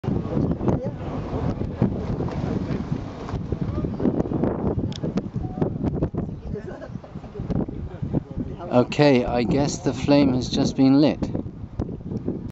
12) the lighting of the Olympic flame, 2016
So, if I've got this right, the flame was lit at noon.  But I wouldn't swear to that.  As it turns out, I now believe that I was wrong (go forward three videos to see why I think that).  Taken on Thursday 21/April/2016.